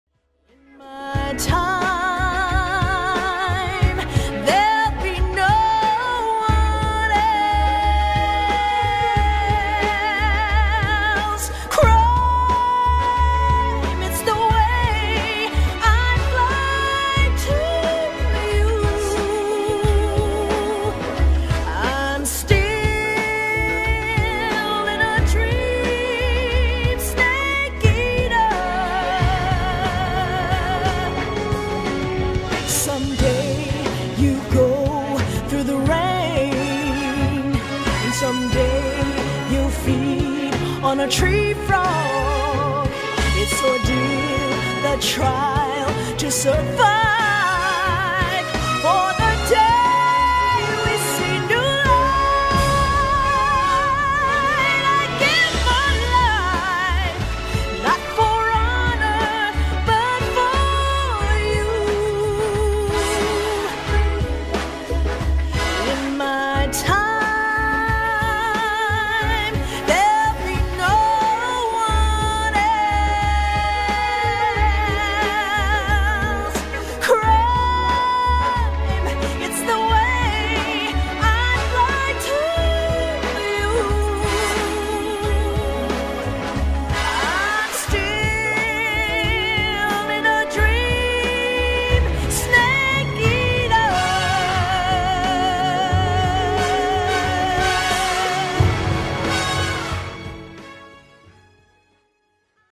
Tornado Yoshida reenacting Revolver Ocelot's gun spinning
I found and edited some footage of Tornado Yoshida reenacting his motion capture work as Revolver Ocelot gun spinning.